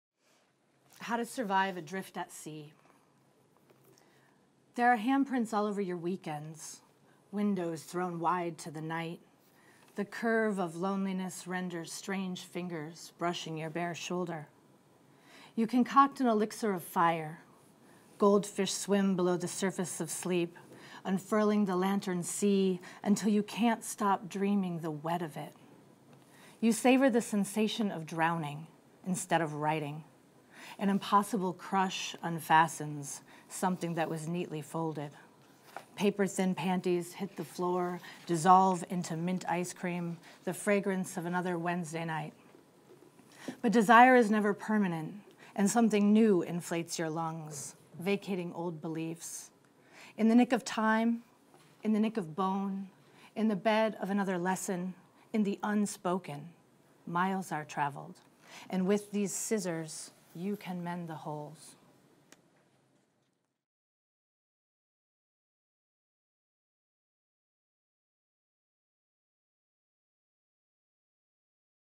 Holly Hardy - The Poetry Center
Hollie Hardy reads “How to Survive Adrift At Sea” from How to Take a Bullet and Other Survival Poems (Punk Hostage Press, 2014), selected for the annual Poetry Center Book Award, on May 5, 2016, at The Poetry Center, San Francisco State University. The full program (see Poetry Center Digital Archive) includes Hardy’s complete reading and a reading by award judge Mukta Sambrani.